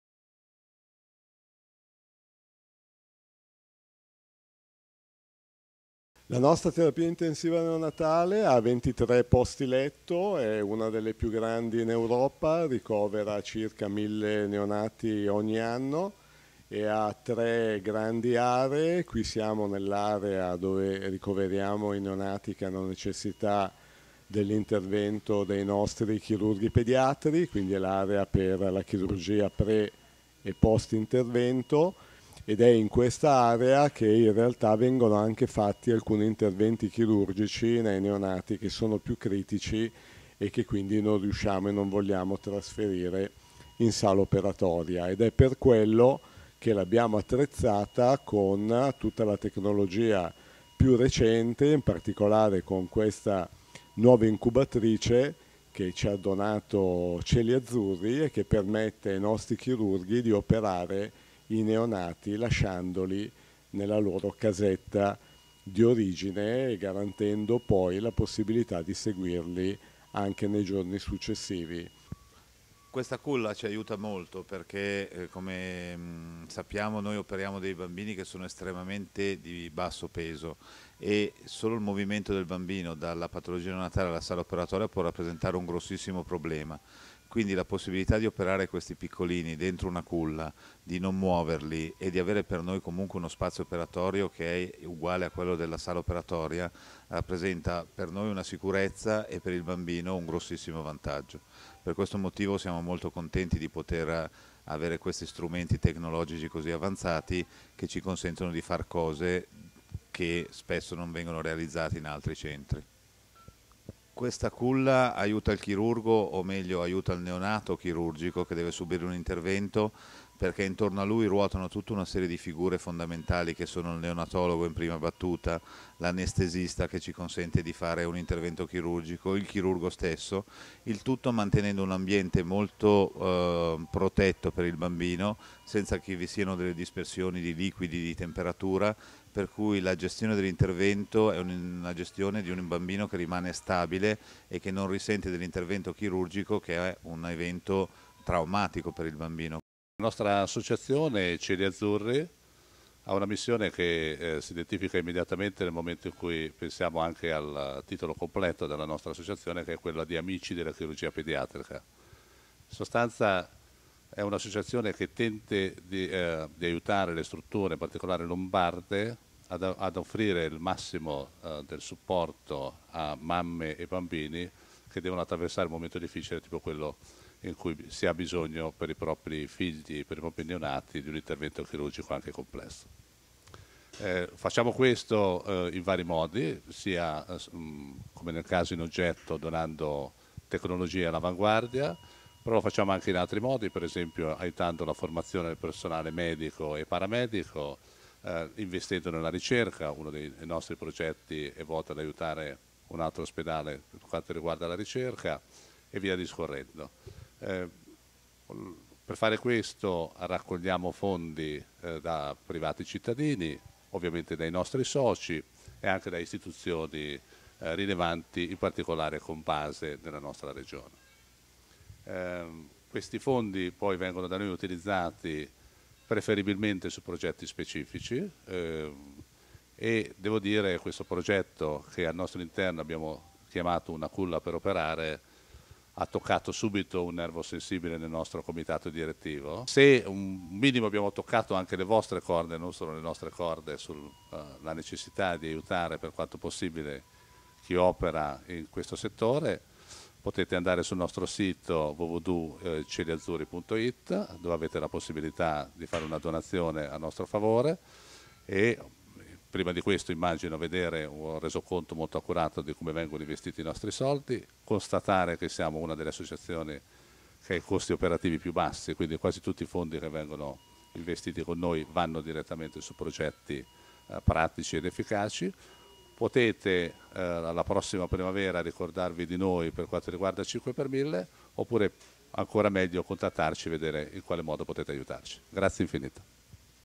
La Terapia Intensiva Neonatale e la nuova 'culla hi-tech' del Policlinico di Milano
Grazie a una donazione di Cieli Azzurri Onlus, la Terapia Intensiva Neonatale (TIN) della Fondazione Ca' Granda Policlinico di Milano ha ora a disposizione una nuova culla 'hi-tech', con la quale è possibile operare i bambini nati molto prematuri con la maggiore sicurezza possibile.
Nel video descrivono l'attività della TIN e della chirurgia pediatrica della Fondazione:
Fabio Mosca, direttore Neonatologia e Terapia Intensiva Neonatale
Ernesto Leva, direttore Chirurgia Pediatrica
Corrado Ruffini, presidente Cieli Azzurri Onlus